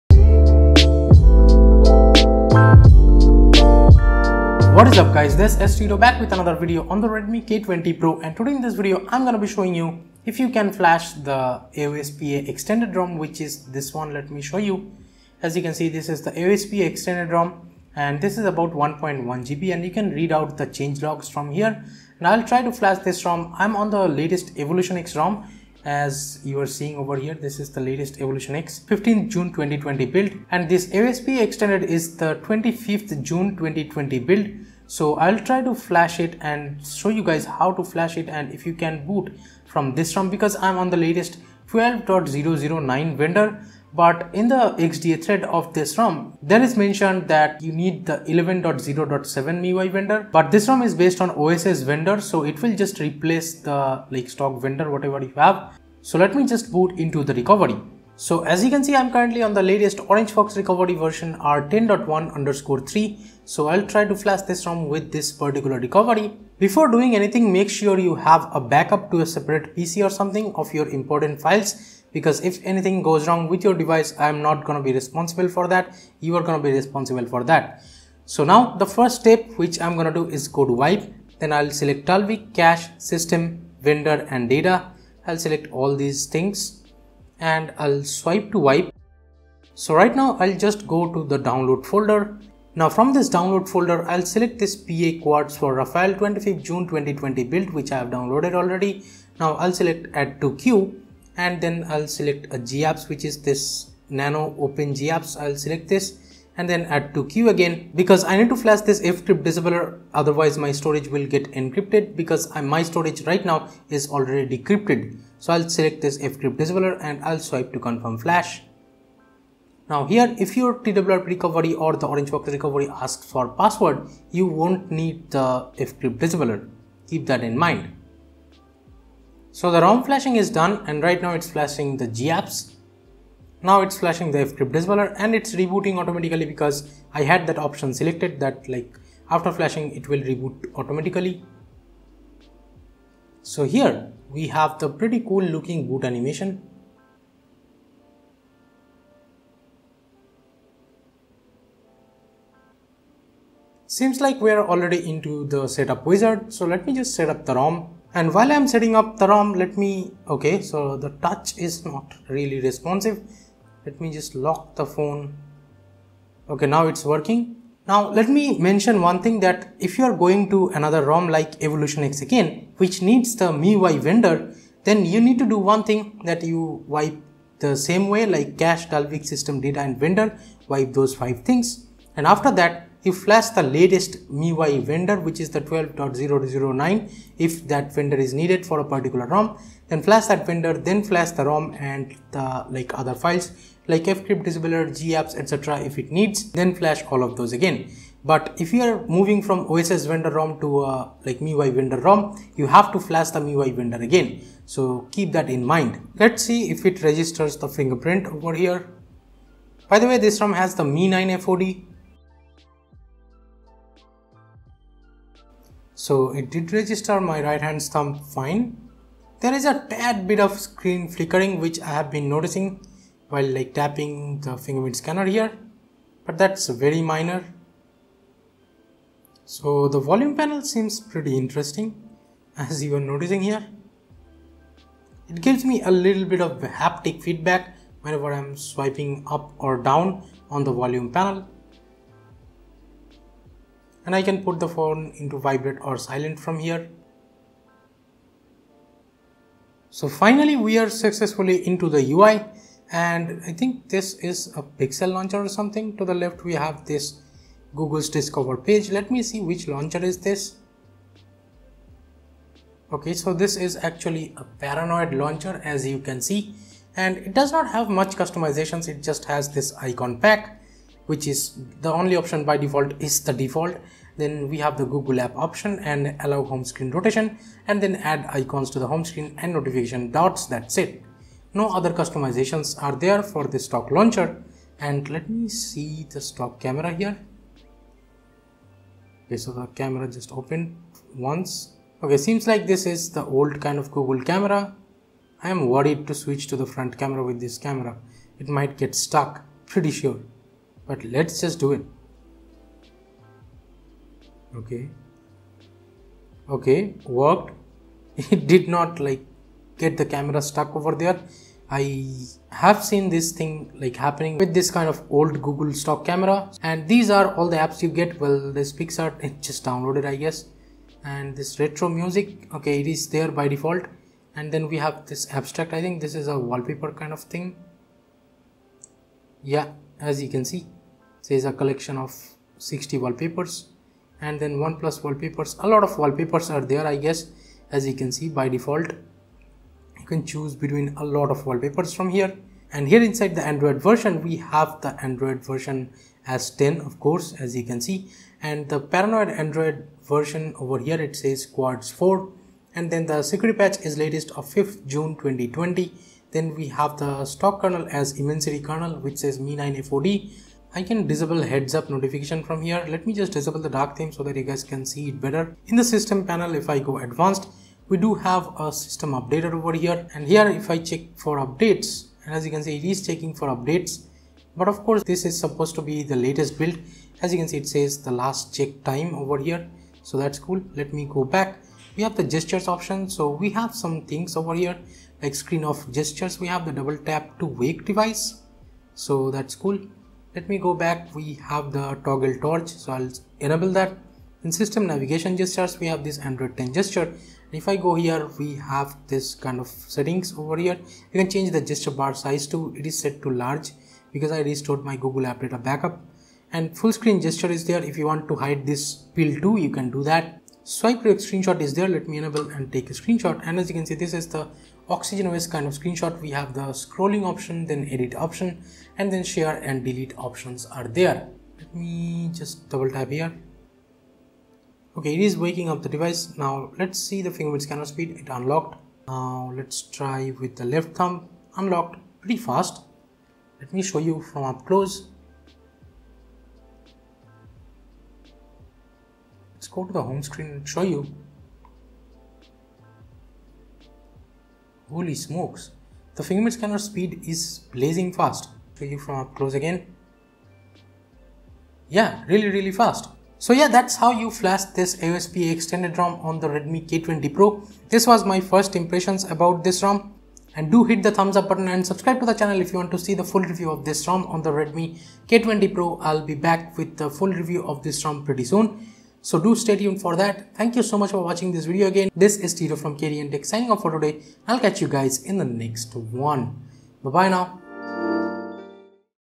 How To Flash AOSPA Extended [OSS Vendor] On Redmi K20 Pro!
UPI: titash.sharma-1@okaxis

Video Gear:
Canon 200D
Boya M1
Zoom H1n
Maono Lav Mic

Video Lighting:
LED Tube Lights

0:00 Intro
0:15 Rom Info
0:58 Flashing
2:33 Setting Up The Rom & First Impressions
4:09 Stock Launcher
4:44 Stock Camera
5:24 Default Wallpaper App
5:42 About Section
6:05 System
7:03 FOD Speed Test
7:33 Outro